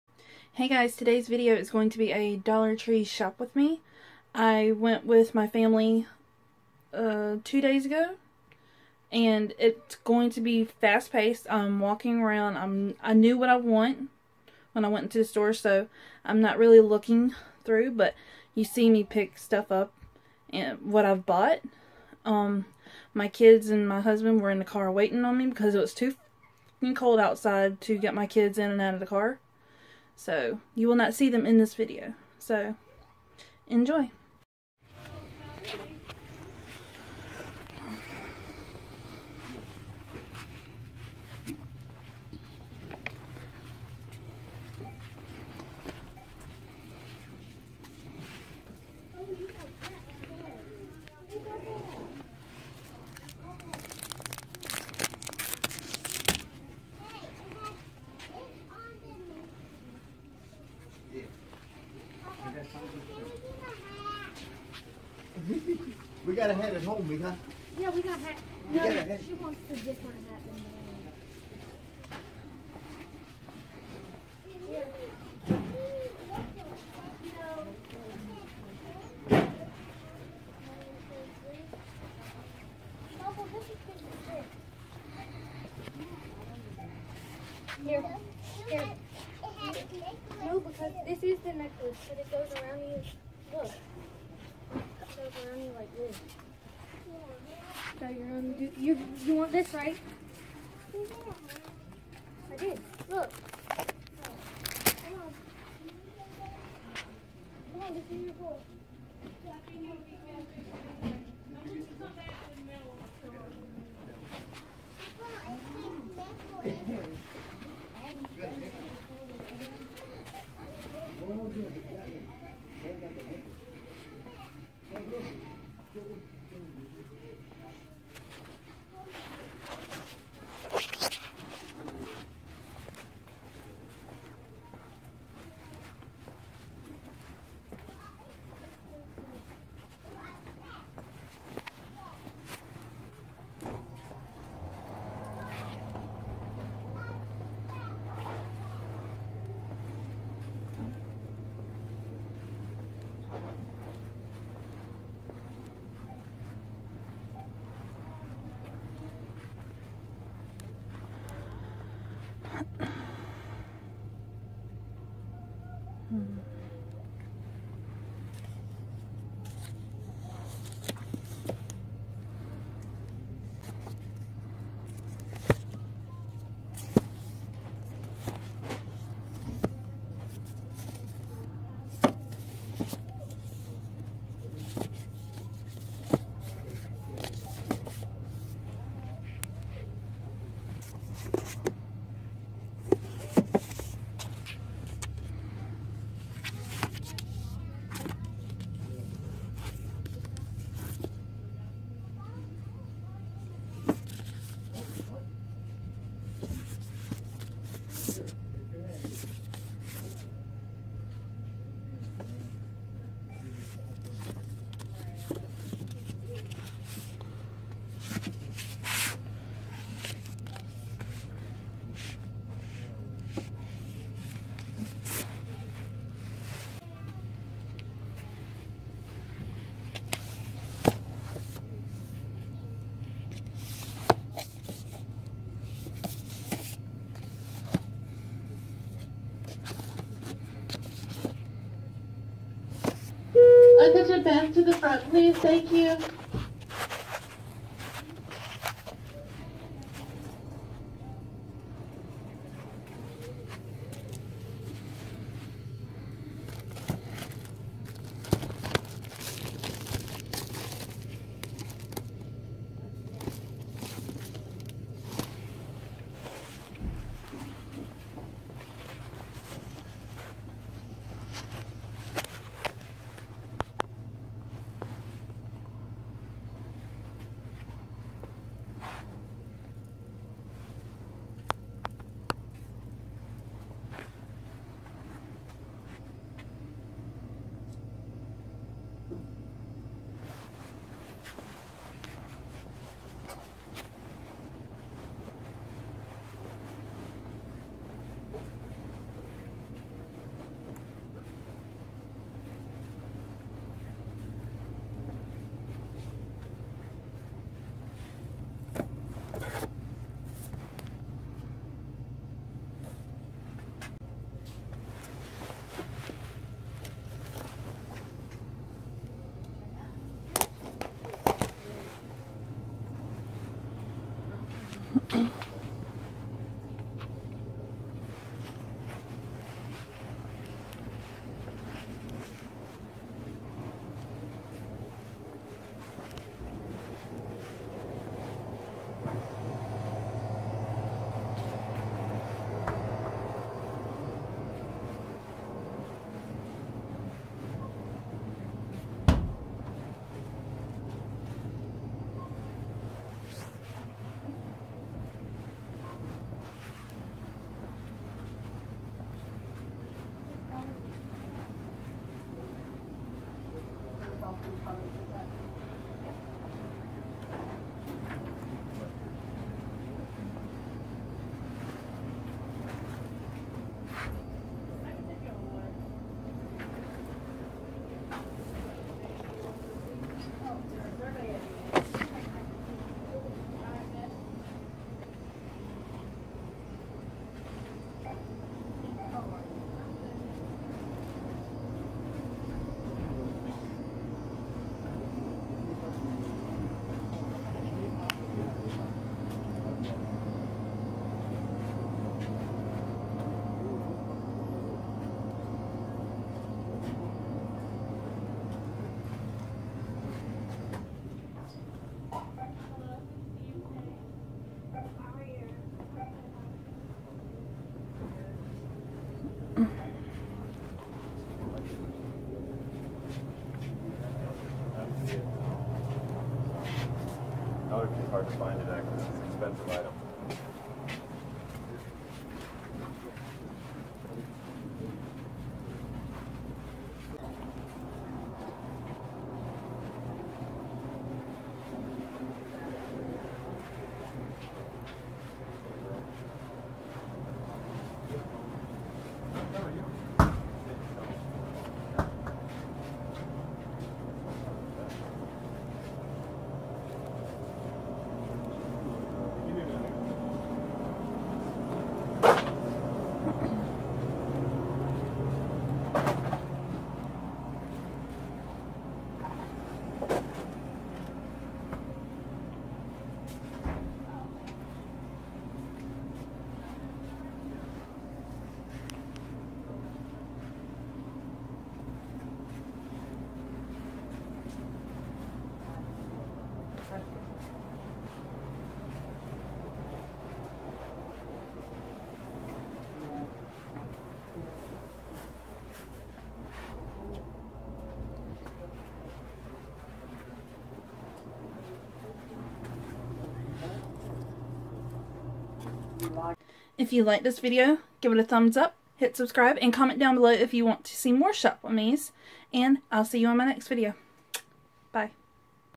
Shop With Me: Dollar Tree
I take ya'll shopping with me to the Dollar Tree. I will be doing a separate video showing the items I bought.